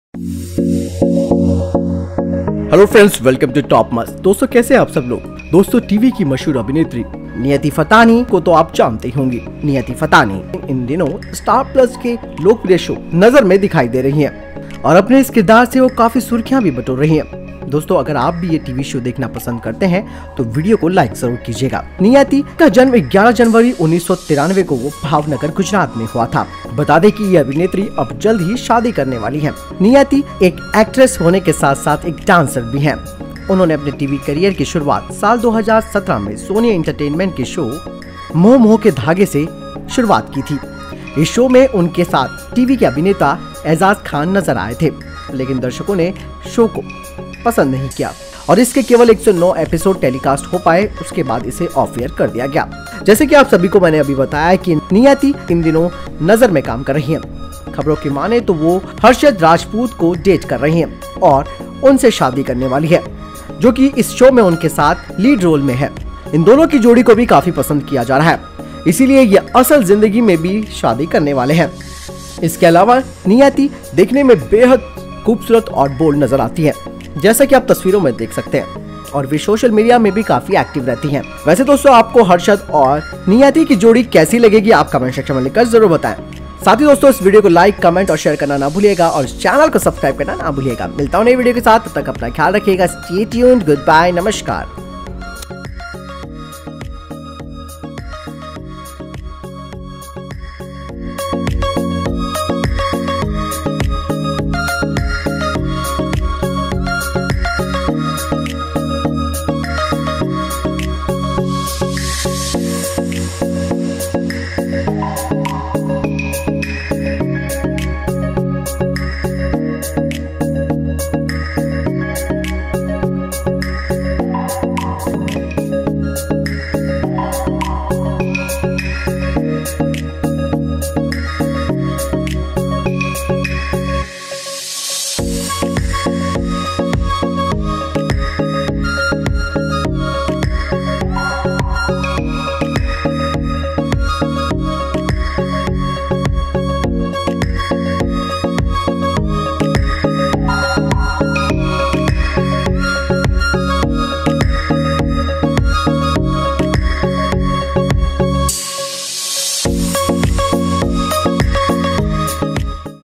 सीरियल नजर की प्रिया असल जिंदगी में बेहद खूबसूरत है। जल्द कर रही है शादी - actress Niyati Fatnani
Niyati Fatnani is an Indian television actress. She is known for television shows Nazar. She started her career with the television show D4-Get Up and Dance in 2016.

Niyati Fatnani was born on 11 January 1991 in Rajkot, Gujarat. She was fond of acting and dancing since childhood. During a stage program, choreographer of D4-Get Up and Dance suggest her to give audition for the show. In her very first audition, she was selected as Niharika Sinha in this TV show. After this show, Fatnani was also seen in Sony TV show Yeh Moh Moh Ke Dhaagey. Fatnani gained fame in the television industry from the 2018 TV show Nazar. In this show, she played an important character of Piya Sharma along with Bhojpuri actress Monalisa.

    Niyati Fatnani was born and brought up in Rajkot.

    She belongs to a Siddhi family.

    She started her career in 2016 with the TV show D4-Get Up and Dance.

    Niyati is a fitness freak and does yoga to keep herself fit.